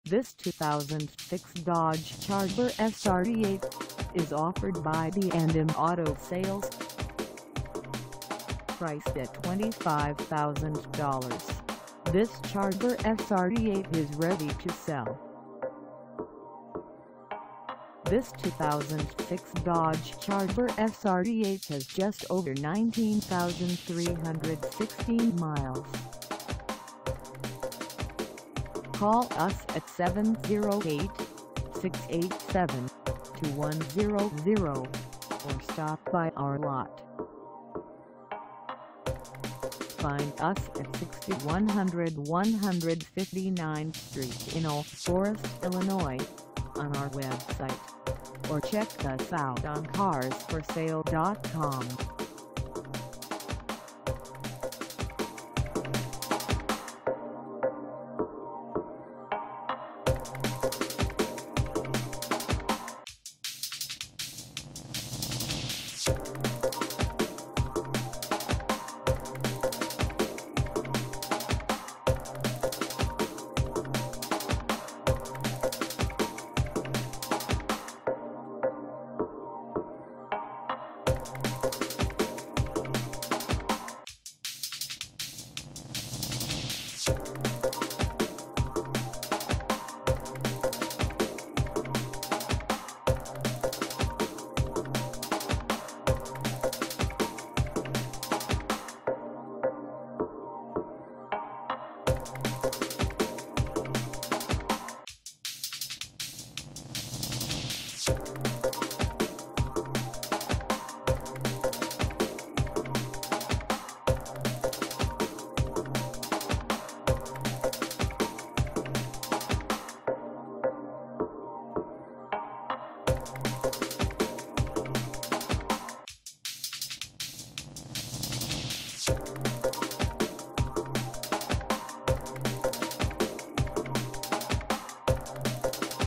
2006 Dodge Charger SRT-8 for sale in Oak Forest, IL 60452 a
This 2006 Dodge Charger SRT-8  is for sale in Oak Forest, IL 60452 at B and M Auto Sales.
Engine: V8 6.1L V8
Transmission: Automatic 5-Speed
Color: Black
Mileage: 19316
Contact B and M Auto Sales 6100 159th St Oak Forest, IL 60452 to test drive this 2006 Dodge Charger SRT-8  today.